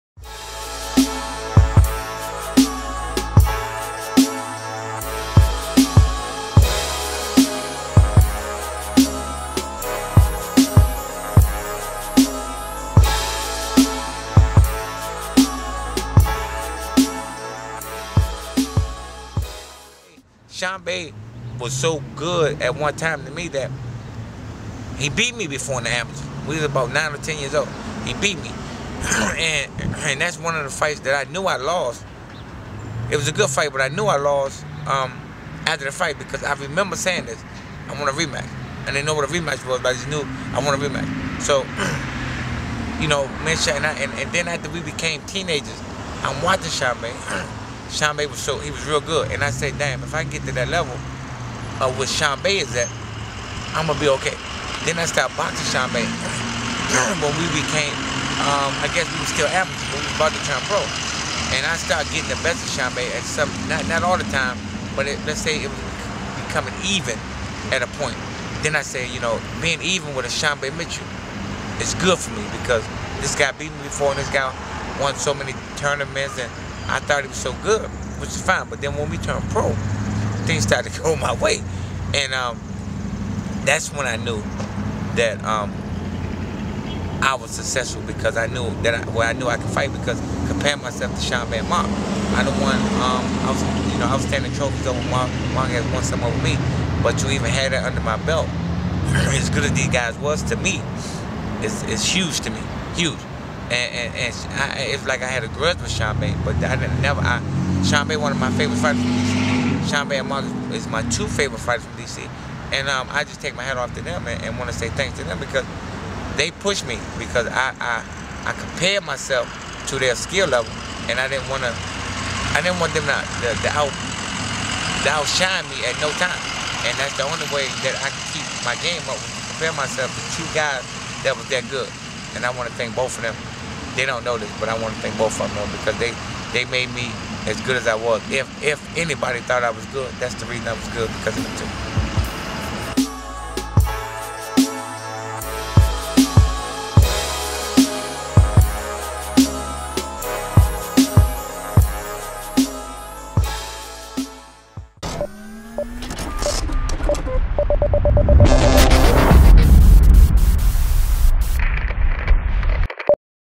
DERRELL COLEY talks SHARMBA MITCHELL
Produced and Directed by @iamStreetLight
4 Time Boxing Champion and Former #1 Ranked Welterweight in the WORLD.  Derrell Coley, DC's Own,  talks Sharmba Mitchell and their quests to be the best from the amateur ranks to the pros.